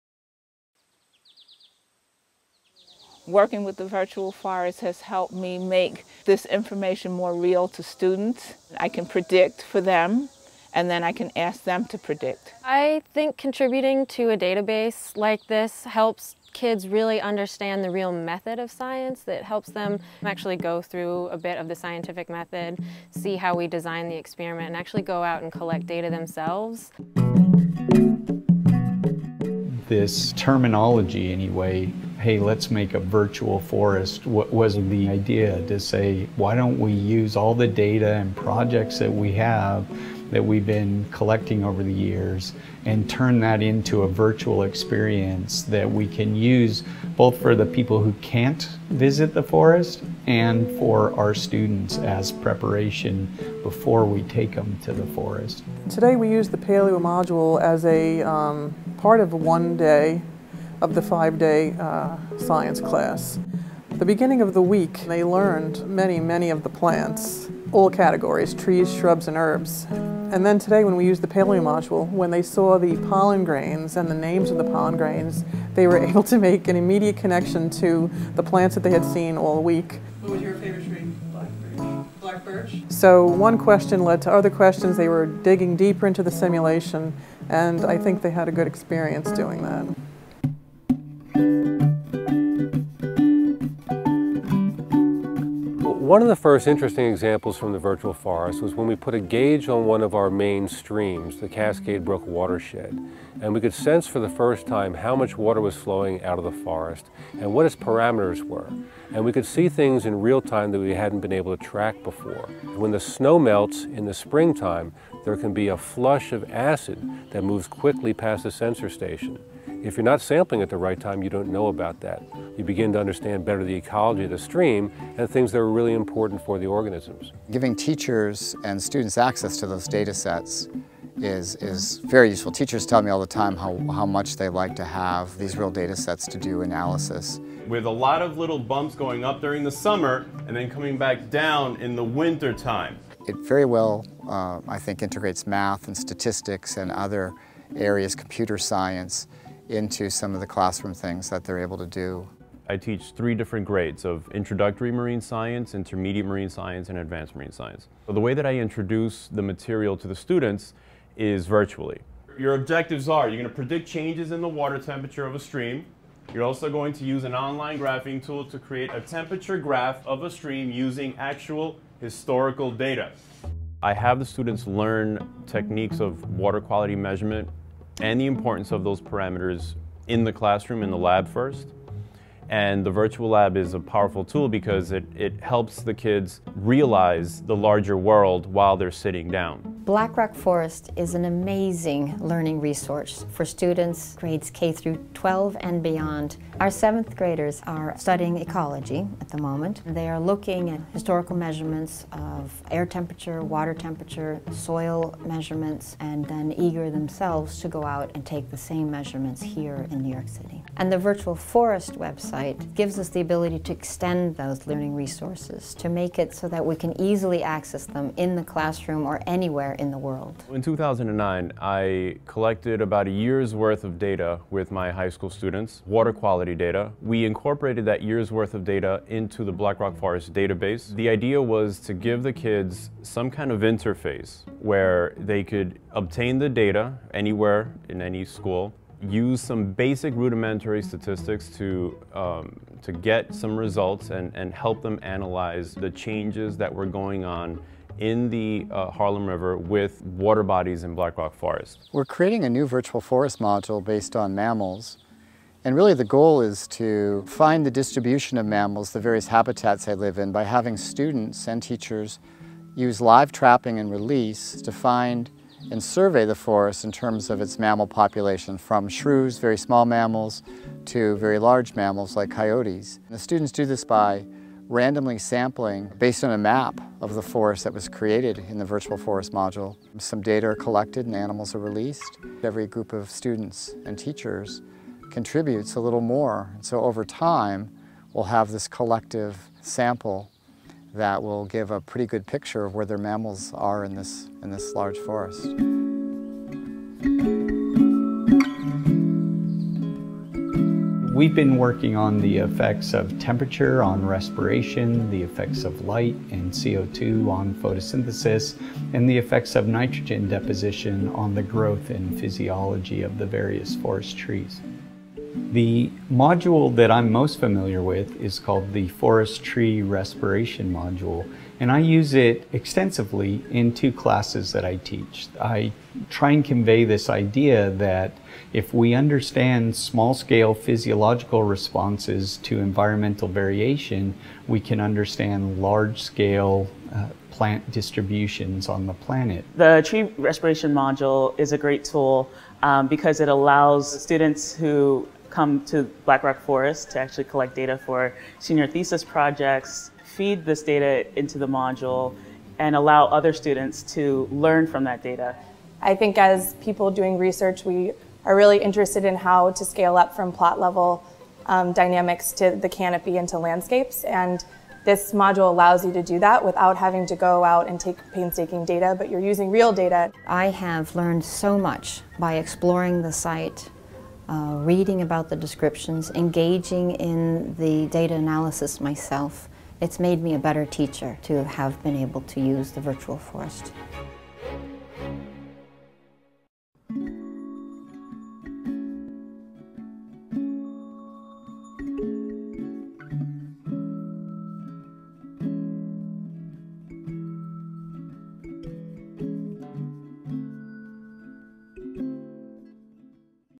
About the Virtual Forest Initiative | The Black Rock Forest Consortium - 2014
The Virtual Forest Initiative supports and enhances research, education, and community activities at Black Rock Forest.  The forest, located 50 miles north of New York City in the Hudson Highlands, is a 4,000-acre natural living laboratory for field-based scientific inquiry; it is operated by a unique consortium of colleges and universities, public and independent schools, and scientific and cultural institutions.

As part of the larger Virtual Forest Initiative, Columbia University's Center for New Media Teaching and Learning developed a suite of web-based learning tools that utilizes Black Rock Forest's rich archive of scientific records such as climate field station and tree data. Used in the Earth and Environmental Sciences courses at Columbia University and Barnard College, the project gives students an unprecedented opportunity to utilize real data for comparative analysis.

By assembling and coordinating a broad array of information, data, and resources, the Virtual Forest Initiative aims to provide researchers, students, educators, and the public new means for learning about all aspects of a research and community forest.

For more information on CCNMTL's work with the Forest, see:
ccnmtl.columbia.edu/portfolio/field-laboratory/virtual_forest.html